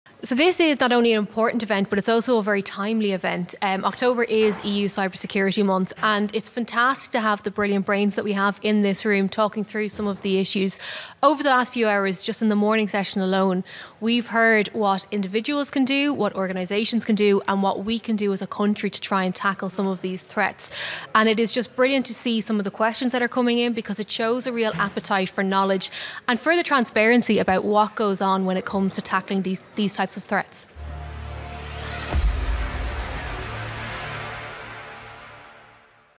Cyber Security Summit 2022: Jess Kelly Interview
Cyber Security Summit 2022

Speaker
- Jess Kelly, Technology Correspondent, Newstalk